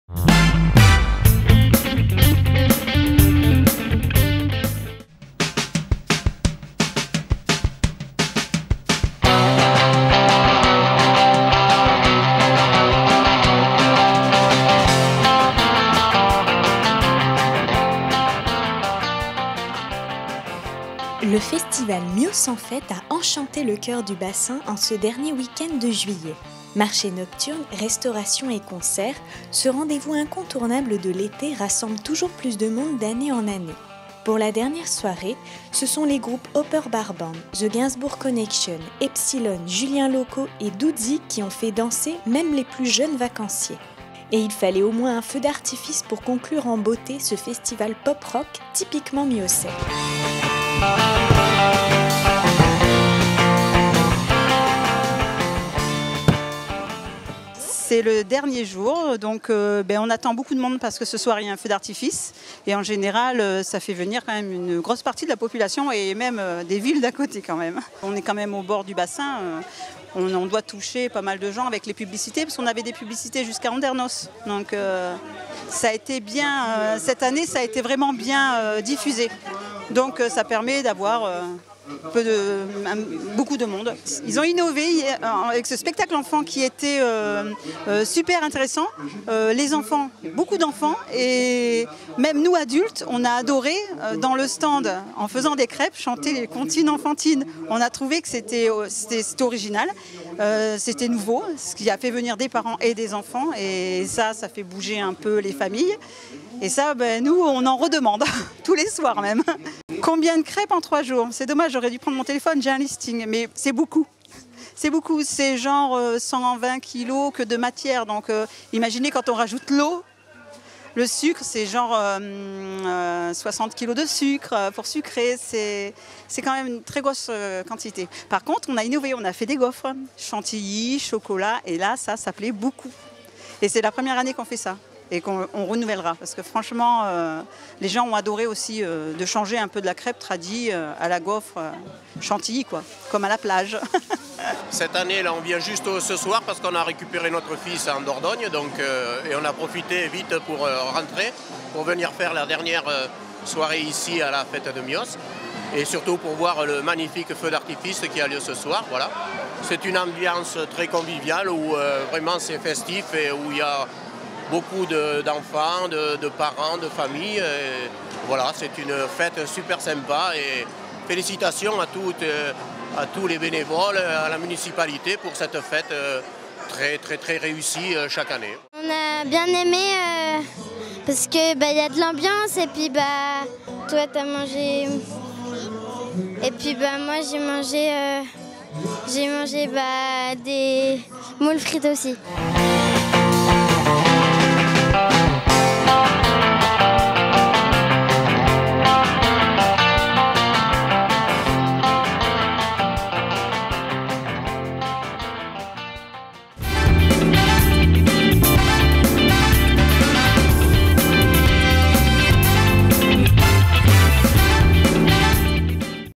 Succès toujours au rendez-vous pour le festival Mios en fête
Le Festival Mios en fête a enchanté le coeur du bassin en ce dernier week-end de juillet.

Marché nocturne, restauration et concerts, ce rendez-vous incontournable de l'été rassemble toujours plus de monde d'année en année. Pour la dernière soirée, ce sont les groupes Open Bar Band, The Gainsbourg Connection, Epsilon, Julien Loko et Doozy qui ont fait danser même les plus jeunes vacanciers. Et il fallait au moins un feu d'artifice pour conclure en beauté ce festival pop rock typiquement miossais.

Interview de Sabine, Bénévole du Comité des fêtes de Mios et de quelques visiteurs